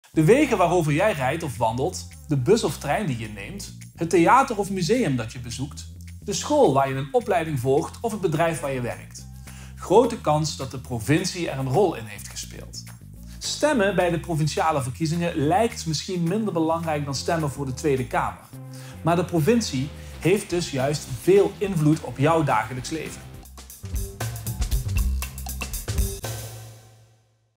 De Provincie heeft invloed op jouw dagelijks leven
Wist je dat de Provincie best veel invloed heeft op jouw dagelijks leven? Sander legt het voor je uit.

15 maart zijn de Provinciale Staten verkiezingen. Meer te weten komen over de verkiezingen of hulp nodig bij het vinden van een partij die jouw stem vertegenwoordigt? Kijk op ikkieslimburg.nl